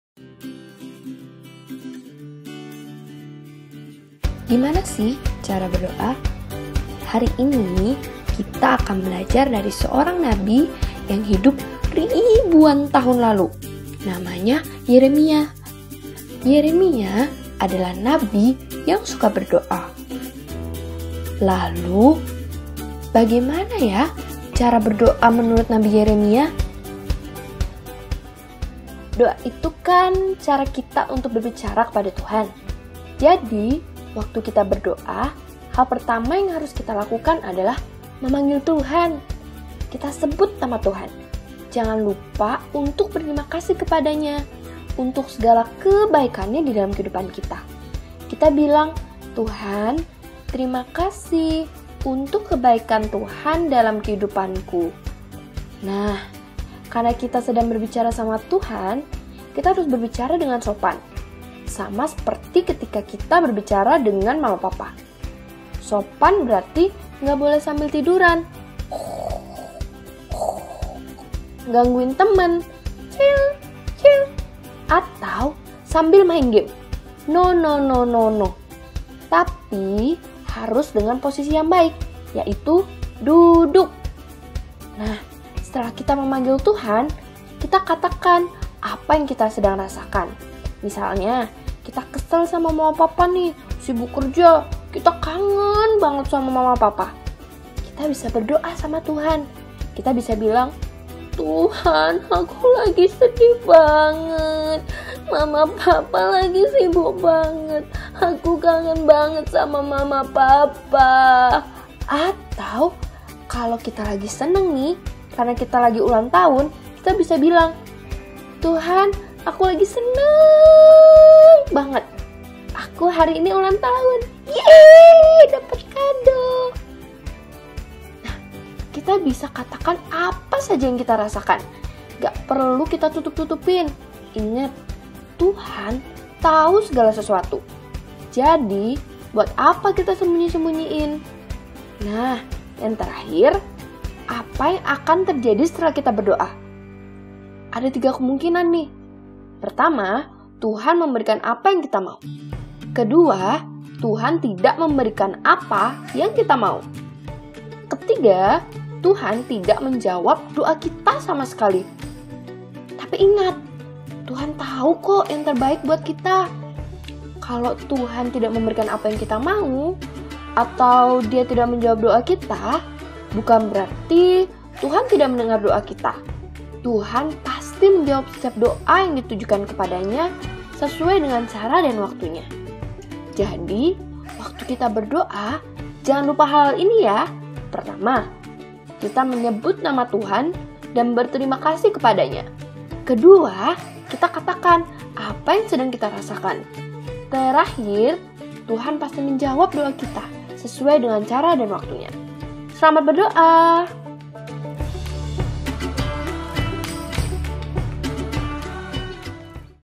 Bagaimana Cara Berdoa? - Jemima Carissa dan Nadia Natasha
Bagaimana cara kita berdoa kepada Tuhan? Sebuah cara berdoa berdasarkan doa-doa nabi Yeremia.